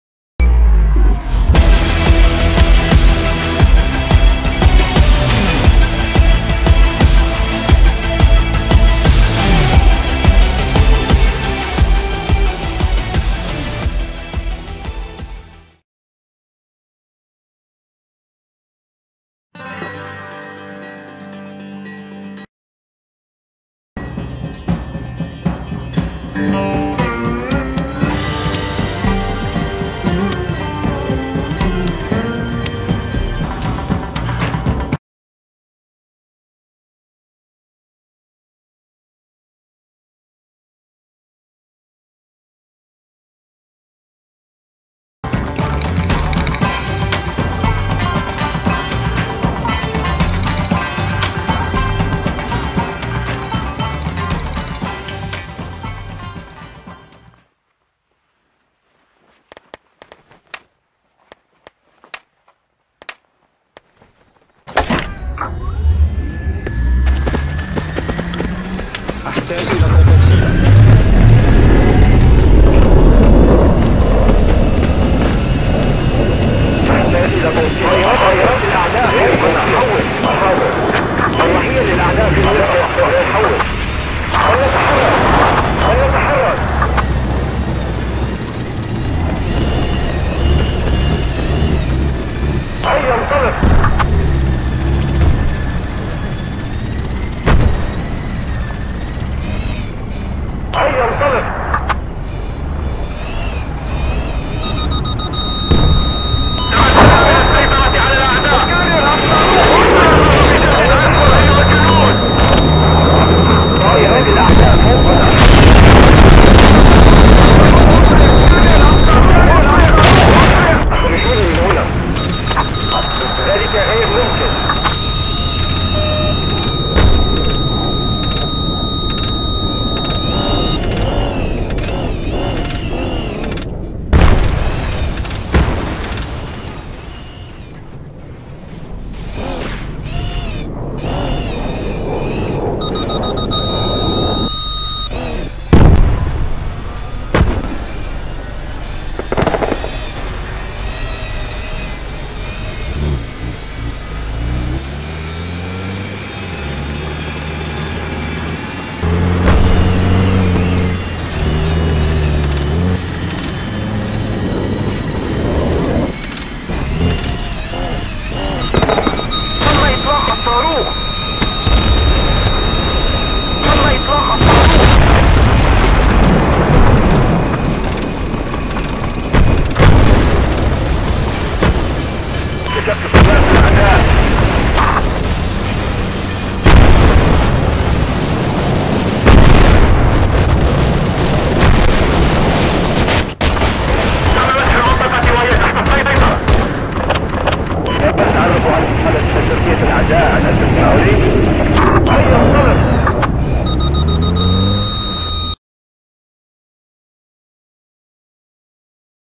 test new map shape of octopus plus vehicle EMZPJ_BF3_VodnikPhoenix
prueba de nuevo mapa y vehículos
vehicles emzpj pack 1,27 gb que tengo de emao en este caso el EMZPJ_BF3_VodnikPhoenix un antiaéreo puedes pedir el enlace de este vehículos y otros de emzpj recuerda registrarte e unirte al grupo escribir ahí en ingles o ruso y disculpa fue corto el video se estrello el juego intentare de nuevo en ventana supongo en algún momento pueda que salga el error aunque últimamente eh agregado mas misiles en gran cantidad o veré de los vehículos últimos o armas pero no creo y tampoco pienso que sea un problema del mapa en fin estaré subiendo mas vídeos gracias por ver este corto